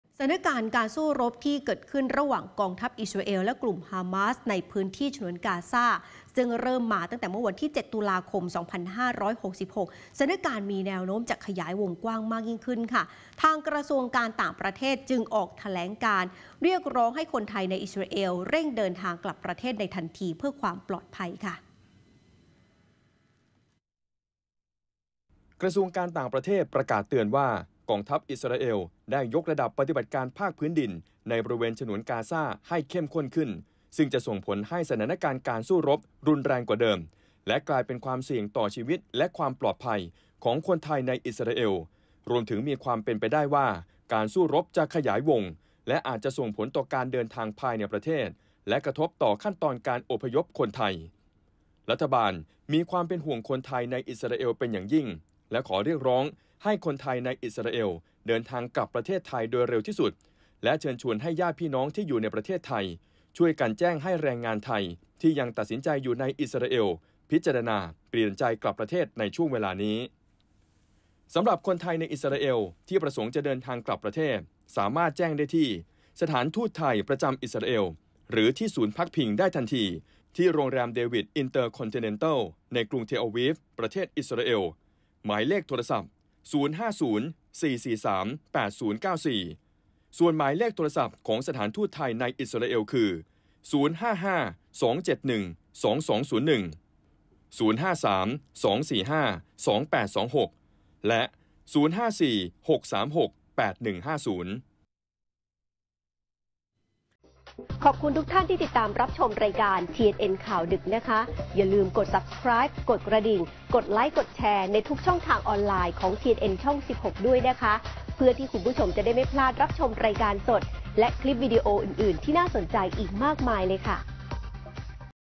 ด่วน! กต.ขอคนไทยกลับทันที ชี้อิสราเอลบุกหนักครั้งใหญ่สุด l TNN ข่าวดึก 28 ต.ค. 66
สถานการณ์ความรุนแรงในอิสราเอล-ฉนวนกาซาที่เกิดขึ้นตั้งแต่วันที่ 7 ตุลาคมที่ผ่านมา ล่าสุดกองทัพอิสราเอลเพิ่มปฏิบัติการภาคพื้นดินในฉนวนกาซามากขึ้น ทำให้กระทรวงการต่างประเทศออกประกาศเตือนคนไทย และเรียกร้องให้เดินทางกลับประเทศไทยทันที